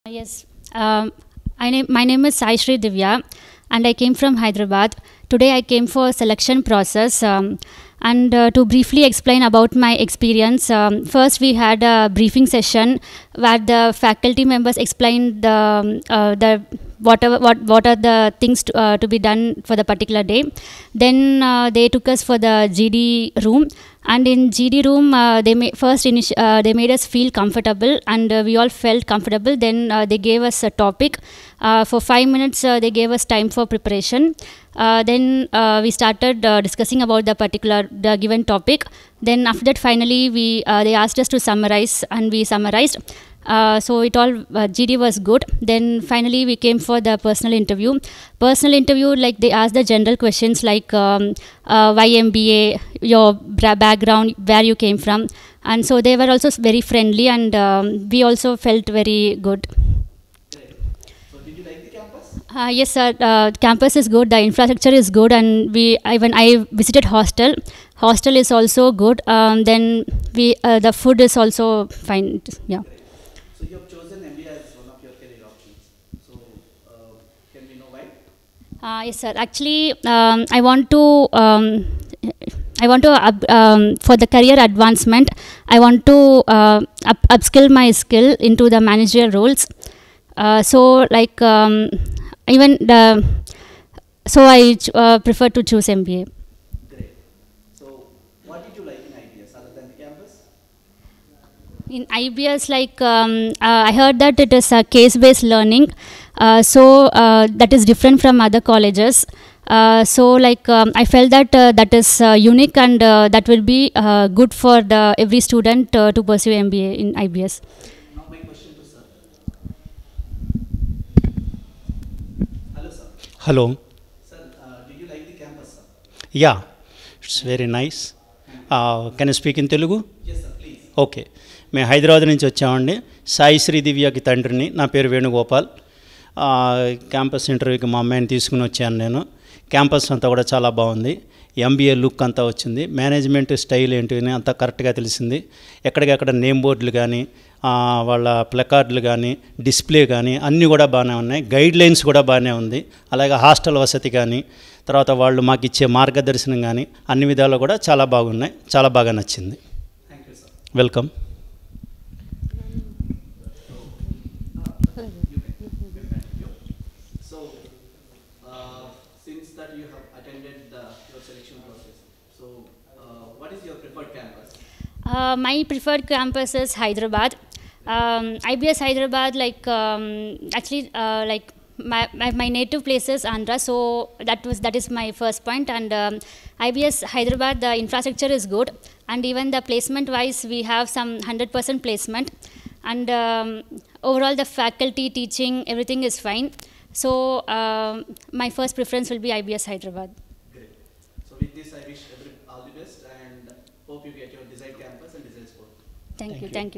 Ms. Sai Sri Divya shared her experience upon completing the IBS Selection process 2024
Ms. Sai Sri Divya a prospective candidate for IBS, provided an in-depth overview of her experience during the IBS selection process in 2024. Her enlightening insights offered valuable perspectives on the journey candidates undertake to become part of the IBS community.